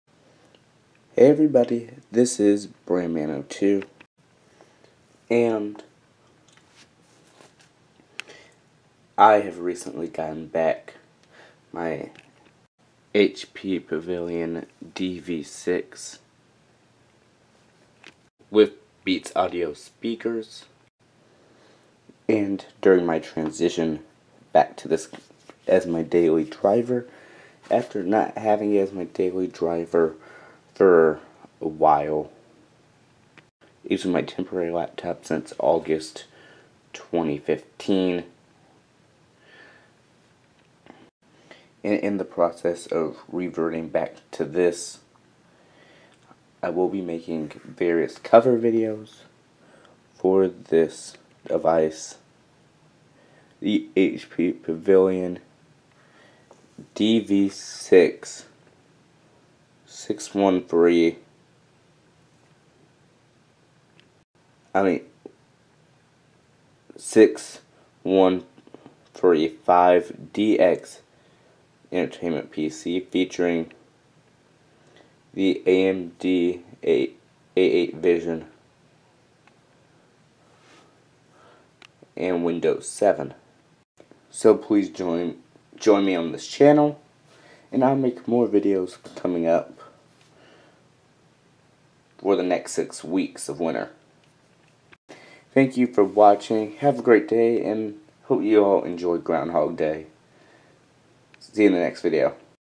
Switching (back) to HP Pavilion dv6 | Statement
Hey everybody, this is BranMan02, and in this video, I just state that I am starting a new series of videos of me switching back to the HP Pavilion dv6-6135dx Entertainment PC as my daily driver. This would be the first time I have used this as a complete daily driver device in over about 2 years. I am pretty busy this week, however, I am going to try to keep my videos coming daily!

Hewlett-Packard belongs to the former company with the same name, as well as HP Inc.
Beats Audio belongs to Apple Inc.